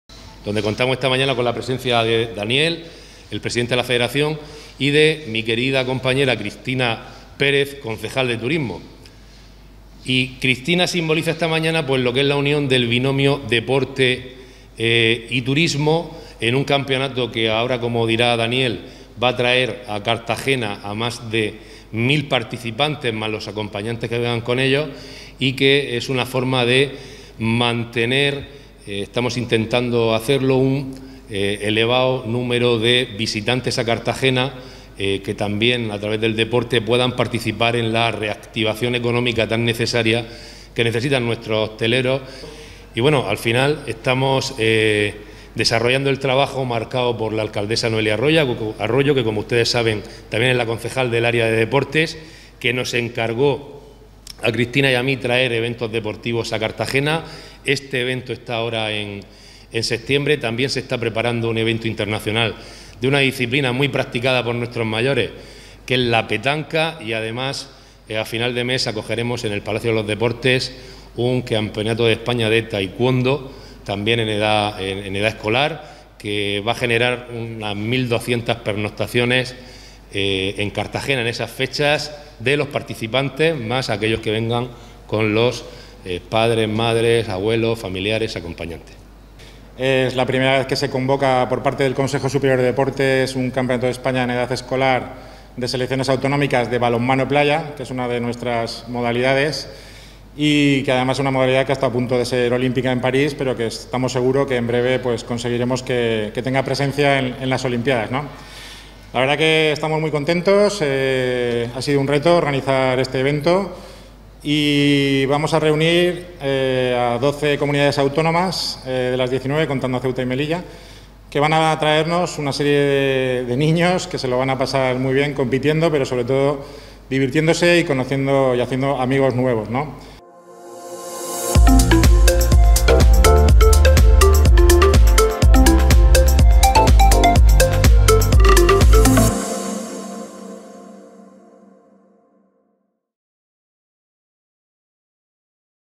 Un millar de jóvenes participarán en La Manga en el Campeonato de España de Balonmano Playa
La tercera edición del Campeonato de España de Balonmano Playa en edad escolar se ha presentado este miércoles, 8 de septiembre, en el Palacio Consistorial de Cartagena. El acto ha contado con la participación del concejal delegado de Deportes, Diego Ortega, la edil de Turismo, Cristina Pérez, y el presidente de la Federación de Balonmano de la Región de Murcia, Daniel Santos.

El evento se desarrolla con el apoyo del Ayuntamiento de Cartagena este fin de semana: los días 10, 11 y 12. Está organizado por la Real Federación Española de Balonmano. Tiene lugar en la playa de Puerto Bello de La Manga. Compiten las mejores selecciones territoriales de esta disciplina deportiva, masculinas y femeninas: Aragón, Madrid, Valencia, Extremadura, Canarias, Castilla La Mancha, La Rioja, Castilla y León, Andalucía, Cantabria, Asturias y la Región de Murcia. Congrega a un millar de participantes de categorías Infantil, Cadete y Juvenil: con edades comprendidas entre los 13 y los 18 años.

APOYO A LA REACTIVACIÓN ECONÓMICA

Ambos concejales han resaltado la importancia que tiene un torneo de estas características para la promoción del municipio de Cartagena y su costa como destino turístico. “Este campeonato sirve también para atraer a un elevado número de visitantes a Cartagena, ya que estos deportistas vienen acompañados de sus familiares. Algo que contribuye a la reactivación económica que necesitan nuestros hoteleros, hosteleros y demás negocios relacionados con el turismo”, han indicado los ediles Ortega y Pérez.

Por su parte, el presidente federativo ha resaltado que “es la primera vez que el Consejo Superior de Deportes convoca un campeonato de selecciones territoriales de edad escolar de balonmano playa”. También ha comentado que se han instalado cinco campos de balonmano en la arena, uno de ellos con graderío para 600 espectadores. Los partidos comienzan el viernes a las nueve de la mañana y concluyen a las tres de la tarde el domingo.